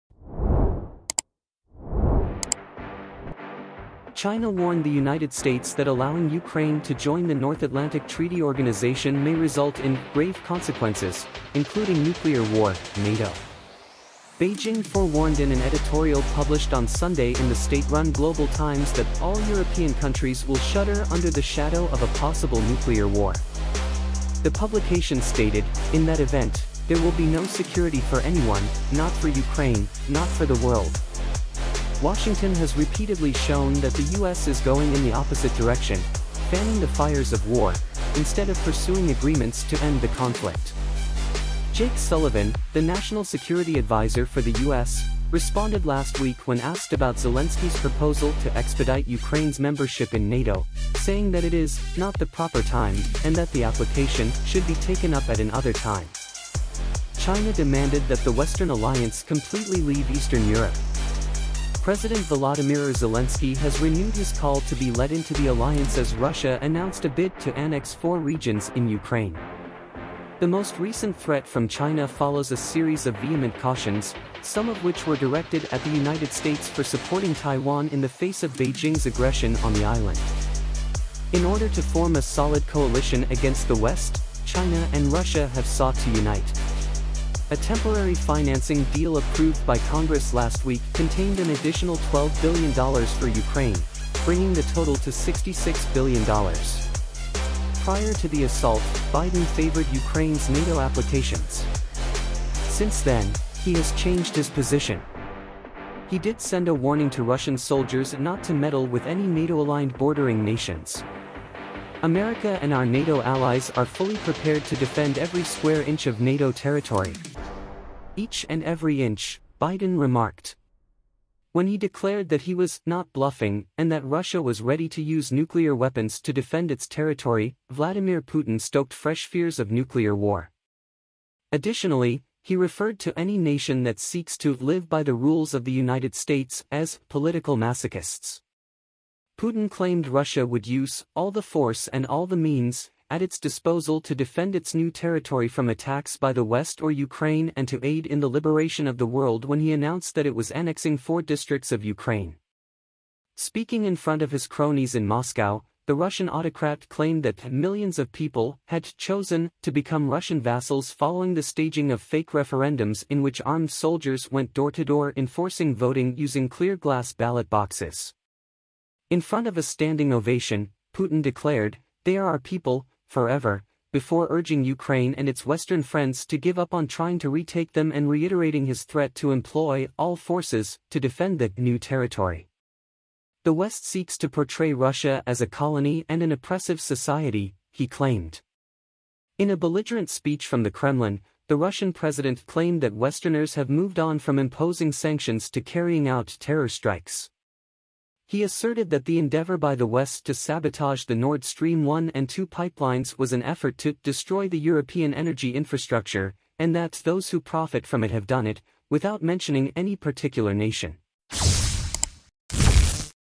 China warns the U.S. of 'grave consequences' - including nuclear war with Russia
China warns the U.S. of 'grave consequences' - including nuclear war with Russia - if Ukraine is allowed to join NATO and more troops are stationed in Eastern Europe